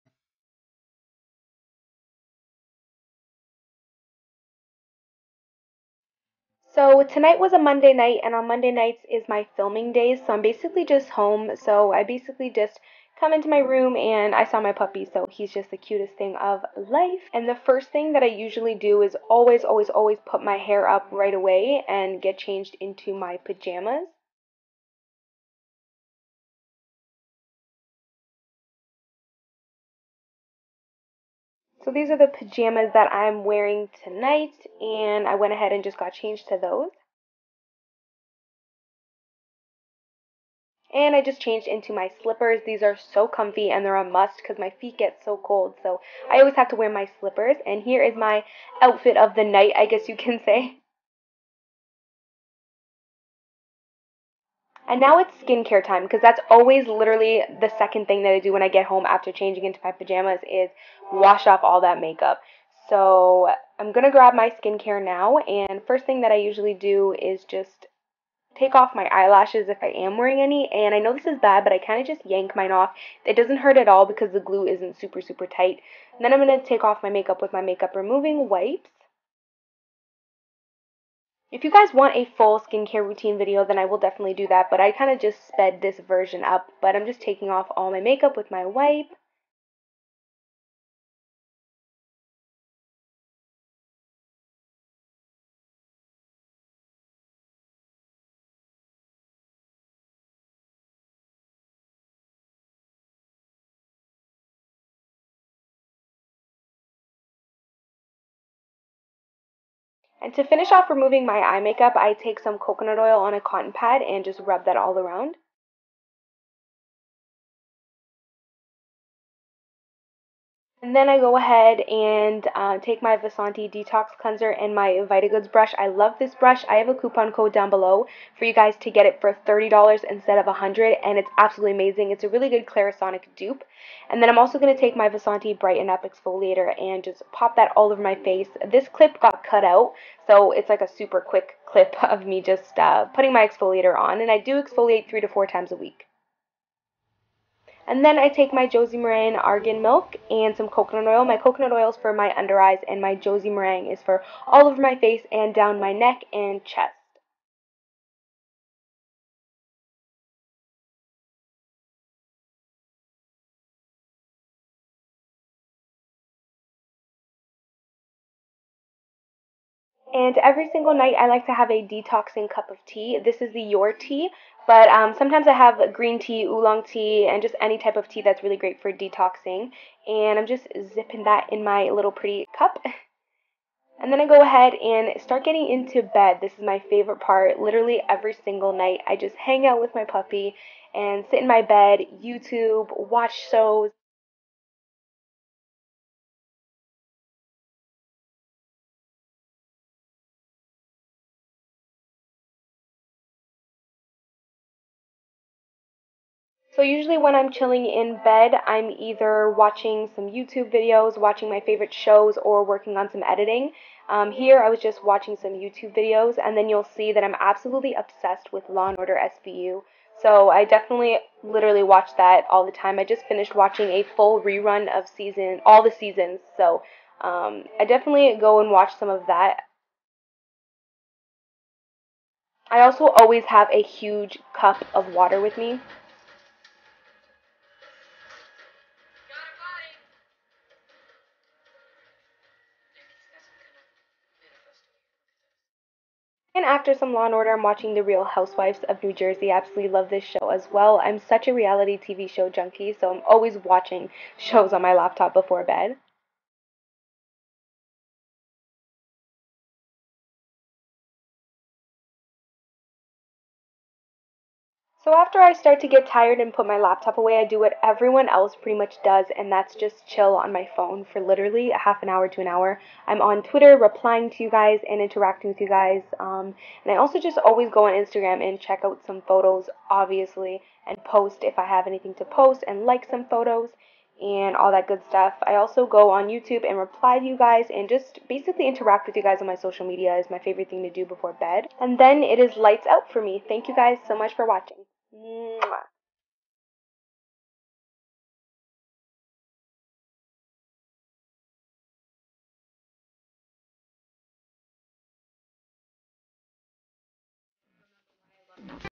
My Night Time Routine
♡ WATCH IN HD & OPEN ME FOR INFO! ♡
I REALLY hope you guys love this video. It was something totally different for me. I filmed it all this morning/afternoon and wanted to get it up ASAP for you. So, I obviously didn't film this at night because I wanted good lighting for the clips, but it's 100% what I do every single night before bed. I'm currently at the part where I'm chilling in bed in my pjs on YT. Lmao! Anyways, this was SO much fun to film and edit. LIKE if you want a Morning routine, or a in depth skincare routine video. xoxo.

♡ Skincare ♡
- Equate Makeup Remover Wipes
- Coconut Oil
- Josie Maran Argan Milk

Makeup: Tutorial coming soon
Clothing: Top - Bebe, Jewelry - Charlette Rouse

Camera: Canon Rebel T3i
Editing: iMovie 11